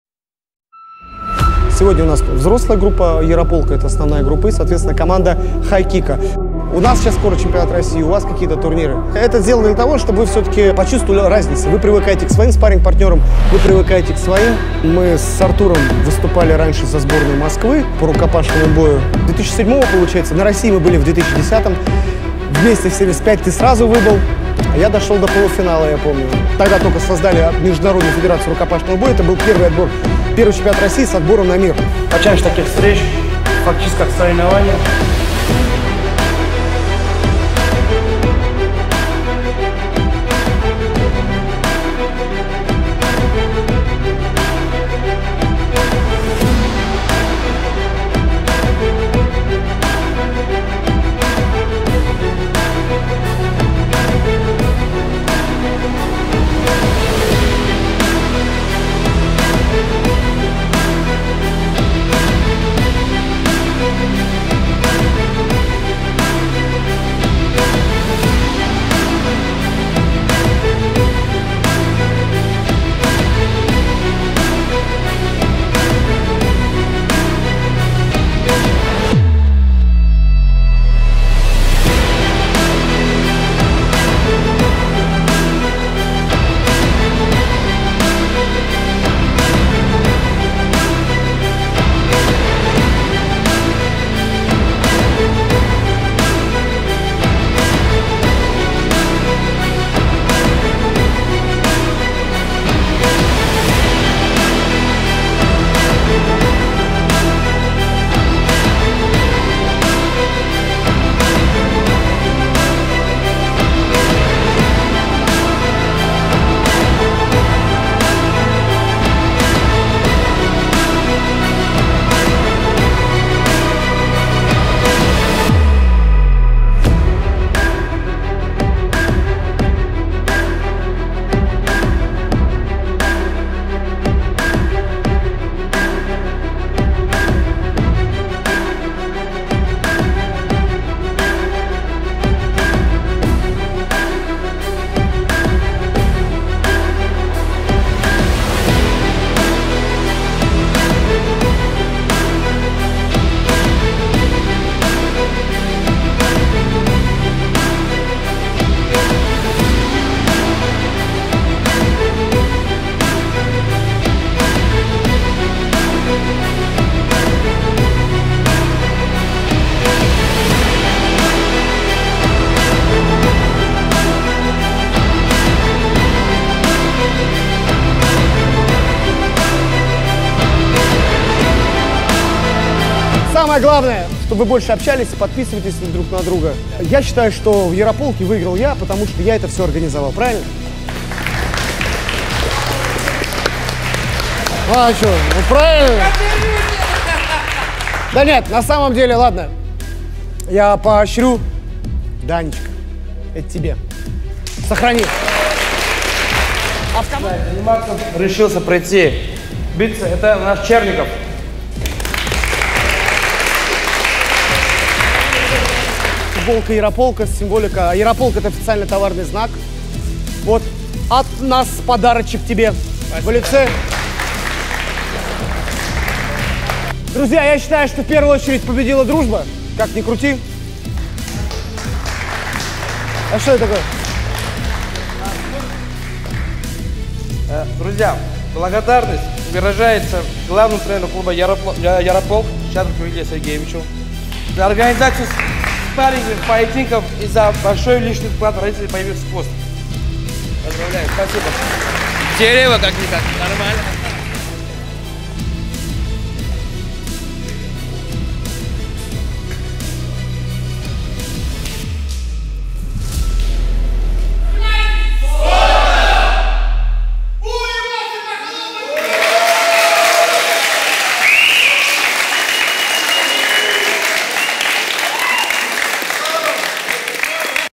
Клуб ЯРОПОЛК (г. Красногорск) vs Клуб HIGH KICK (г. Видное), юноши и мужчины, ММА, Нокаут, март 2021
20 марта 2021 года в СПК «ЯРОПОЛК» состоялась совместная тренировка по смешанным единоборствам спортсменов «ЯРОПОЛКа» с клубом "HIGH KICK" из г.Видное Московской области.
🥊 Во второй части тренировки приняло участие 36 спортсменов от 14 лет и старше.
✅ Огромное спасибо руководству и спортсменам команды "HIGH KIKC" Артур Варданян за насыщенную тренировку.
✅ Данные встречи мы будем делать постоянно с различными секциями, так как на них спортсмены не только приобретают опыт, но и самое главное - находят новых друзей.
💯 Спорт – это биение твоего сердца, твоё дыхание, это твой ритм жизни.